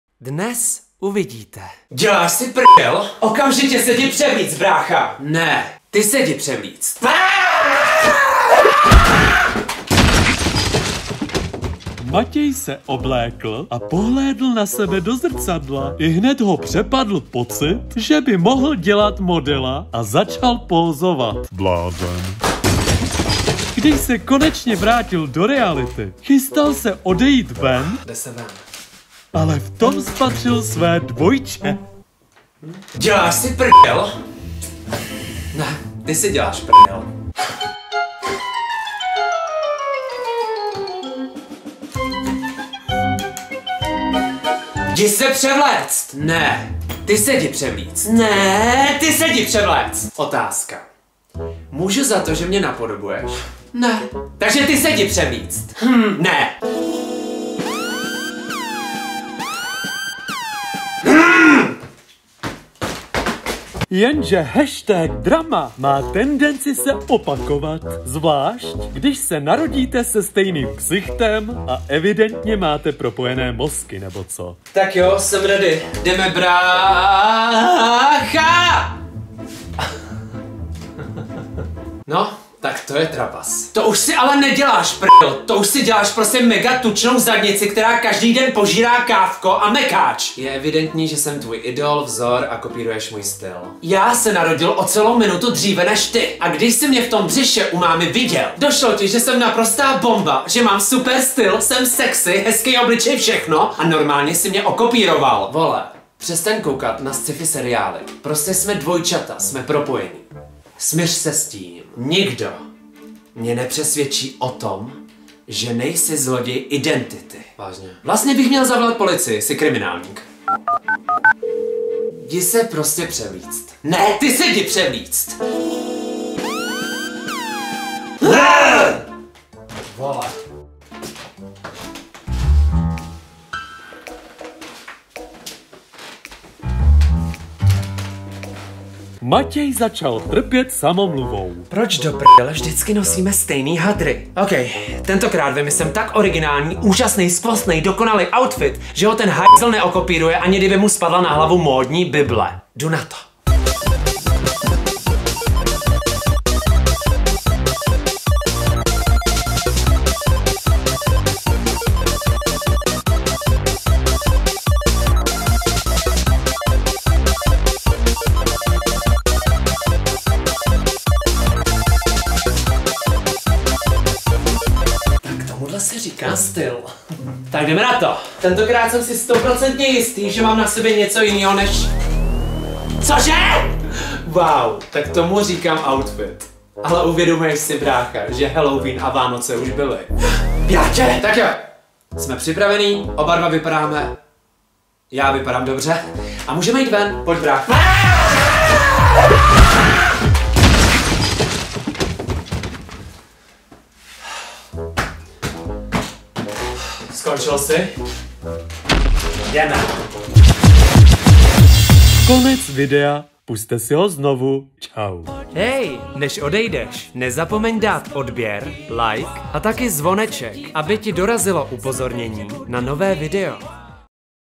Moje dvojče mi ničí život / Otravní spolubydlící
Taky vám sourozenec krade oblečení? Jedním z největších problémů, kterým dvojčata v životě čelí, je bezesporu právě oblečení a kopírování stylu. Co? Vážně jste si mysleli, že napíšu něco moudrého? NE! Je to tak! Brácha mě neustále kopíruje a ještě si z toho dělá srandu. Mám toho dost, a proto jsem se ho rozhodl odhalit celému světu.